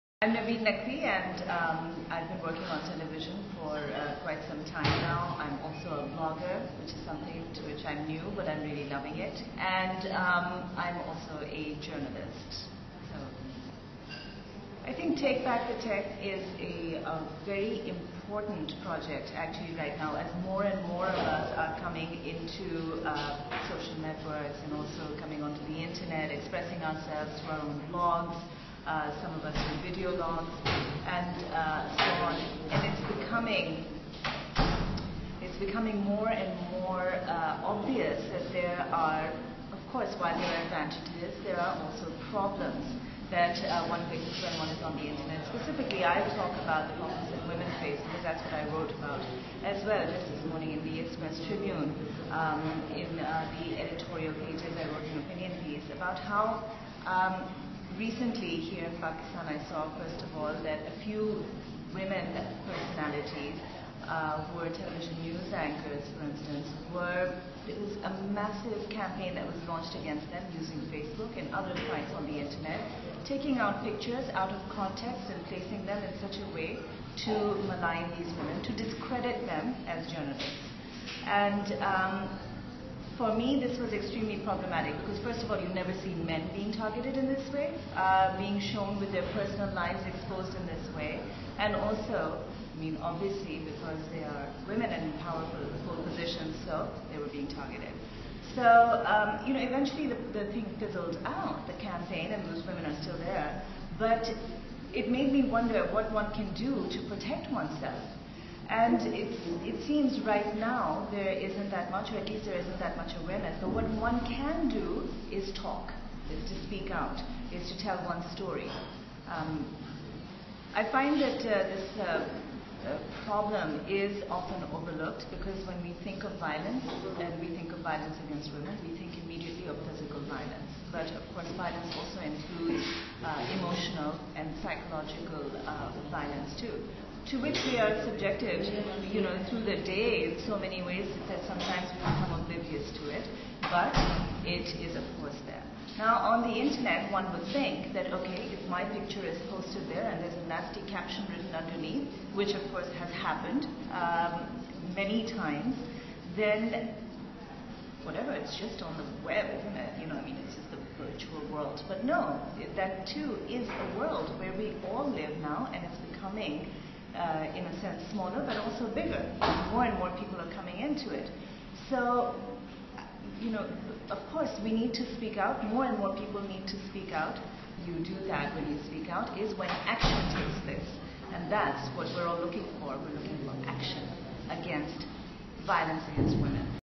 Naveen naqvi about VAW
TV Personality, Journalist & Blogger Naveen Naqvi talks about TBTT & VAW
Category: Nonprofits & Activism